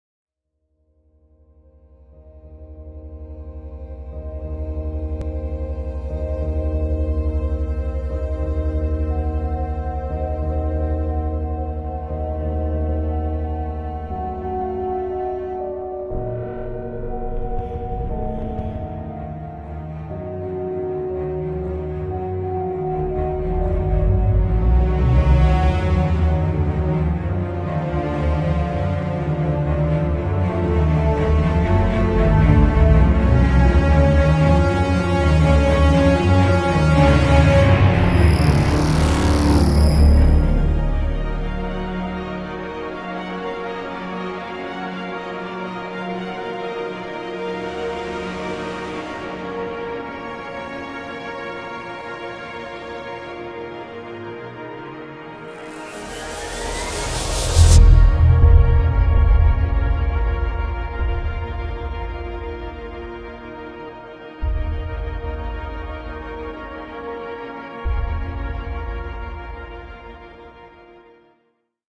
Atmosphere | Spitfire Audio Competition: Start Scoring Movies Now
I rescored this trailer for the spitfireaudio competition